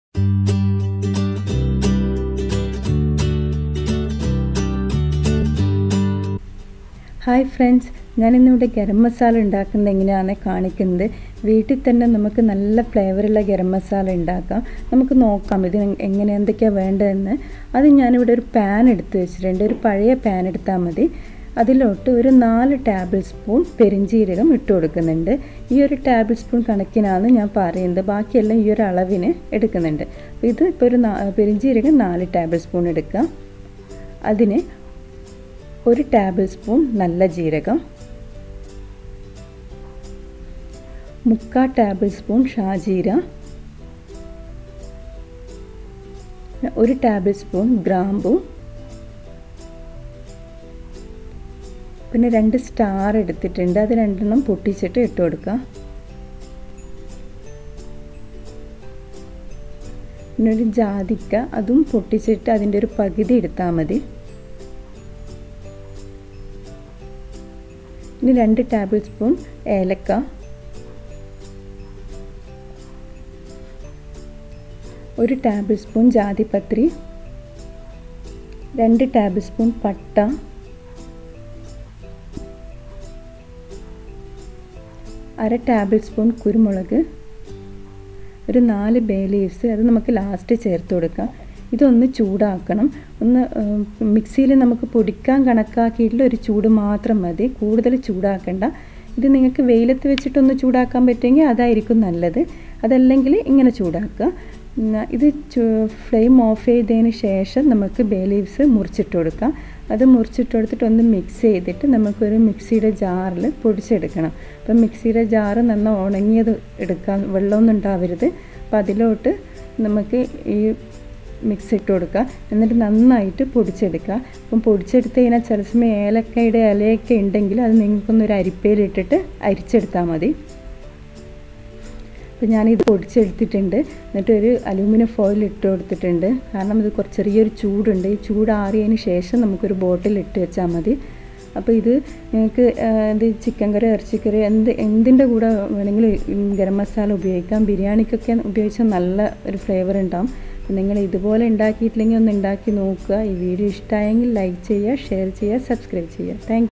Homemade Garam Masala | ഗരം മസാല | Nasha's Kitchen
Ingredients
Shajeera - 3/4 tbsp.
Cloves - 1 tbsp.
Star Anise - 2
Cardamom - 2 tbsp.
Mace -1 tbsp.
Whole nutmeg - 1/2 pc.
Cinnamon sticks - 2 tbsp.
Black pepper corns - 1/2 tbsp.